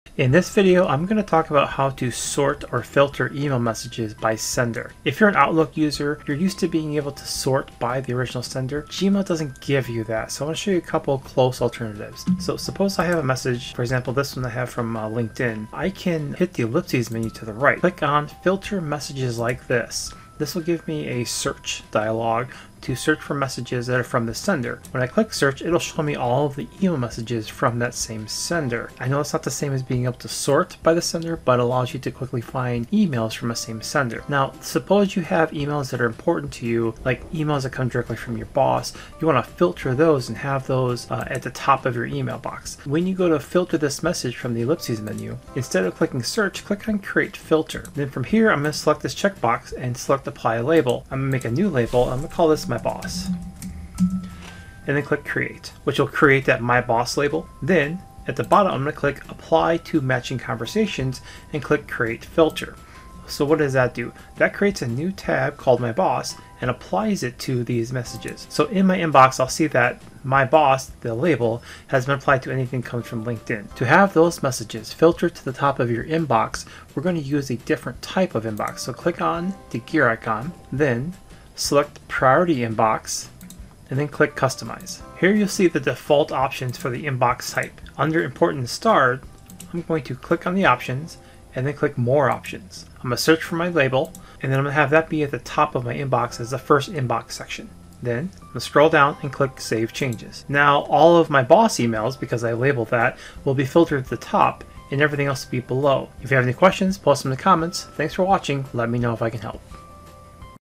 How to Sort Gmail by Sender
Use Gmail filters to filter specific senders to the top of your Gmail inbox.

🎞️ I use Camtasia to record my videos.  Try Camtasia for free

© Bryant Cobarrubias (@bryantcobo)